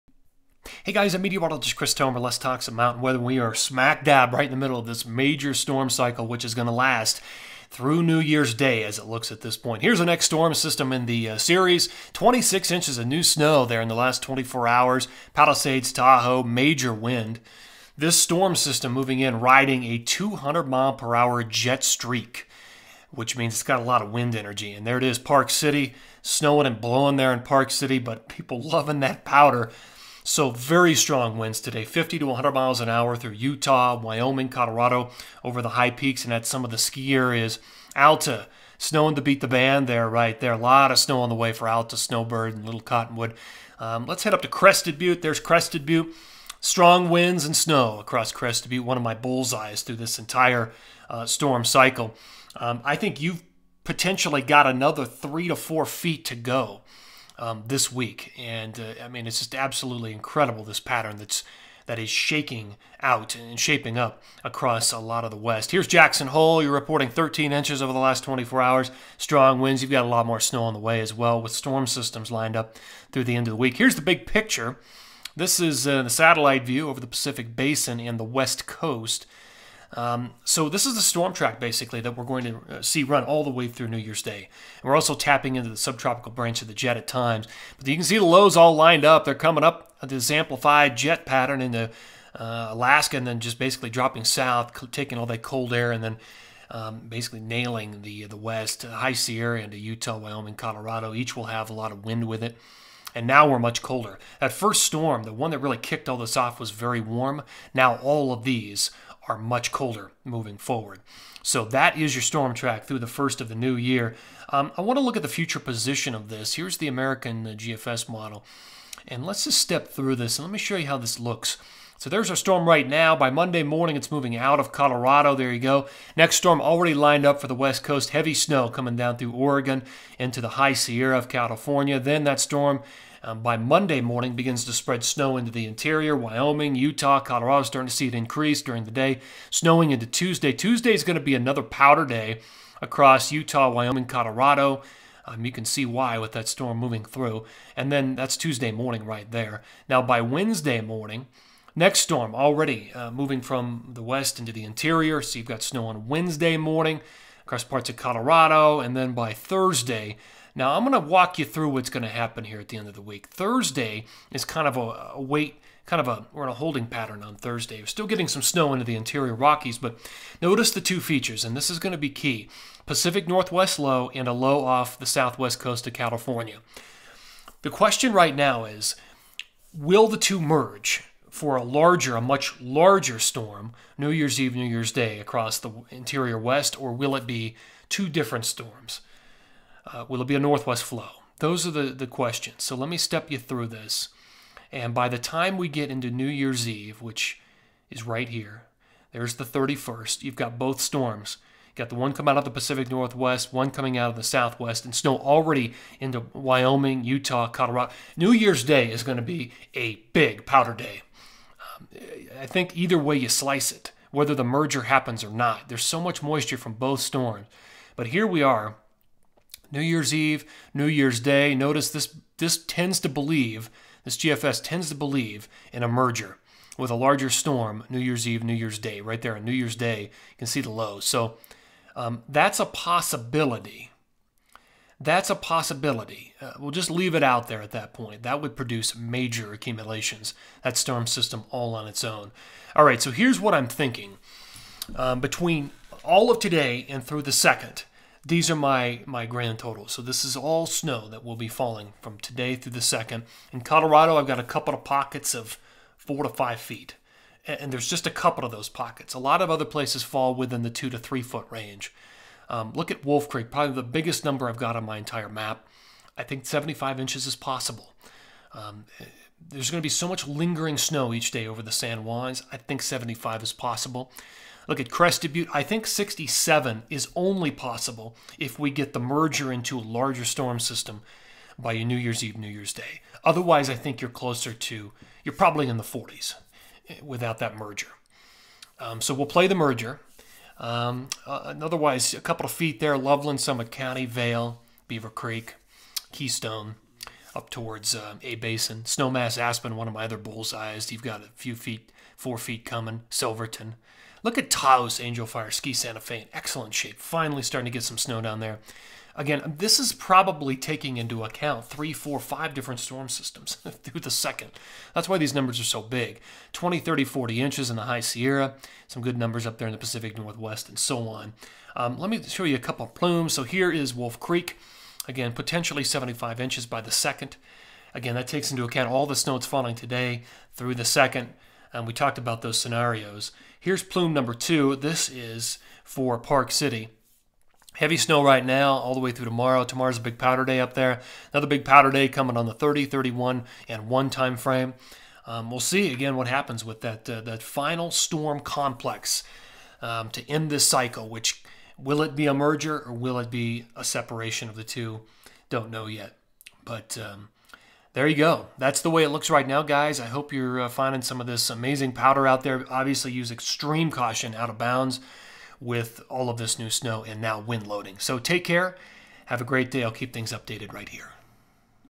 Mountain Weather 12/26, Meteorologist Chris Tomer
Major storm cycle continues into 1/1/2022 with feet of accumulation across the West.